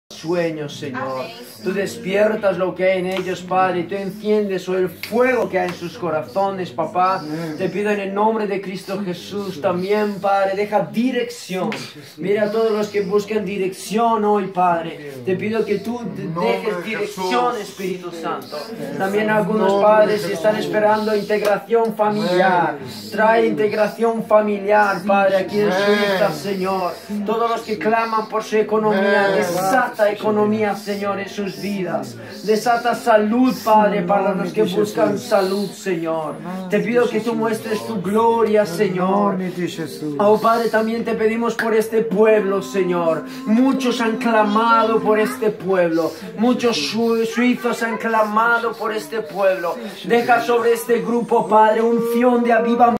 Oración por la casa ministerial y por Suiza. Tour Misionero Unidos Por Europa
Video de Valerio Yendo x La Tierra